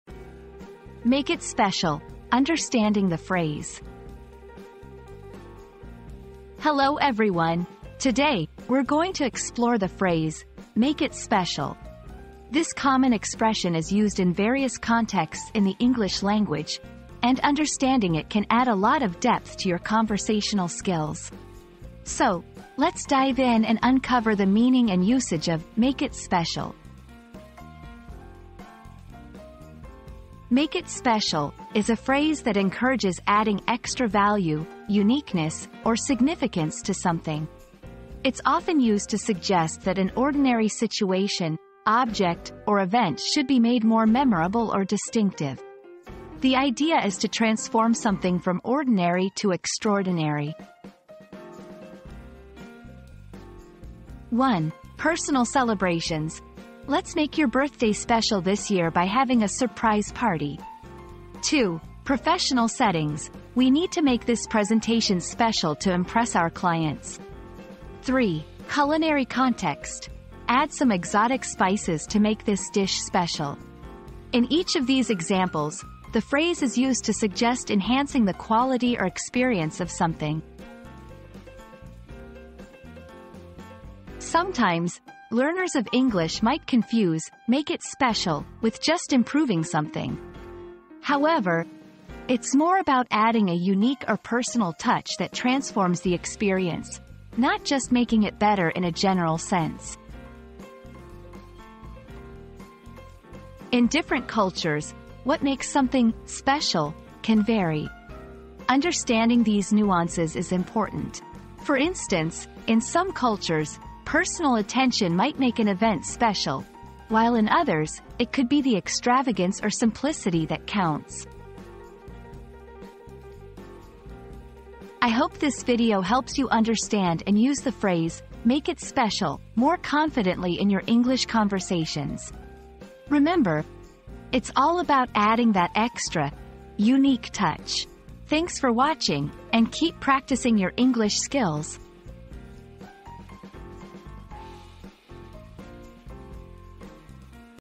Make It Special: Understanding the Phrase
00:00 • Introduction - Make It Special: Understanding the Phrase
00:29 • Defining the Phrase
00:53 • Examples in Everyday Conversation
01:23 • Common Misunderstandings
01:43 • Cultural Nuances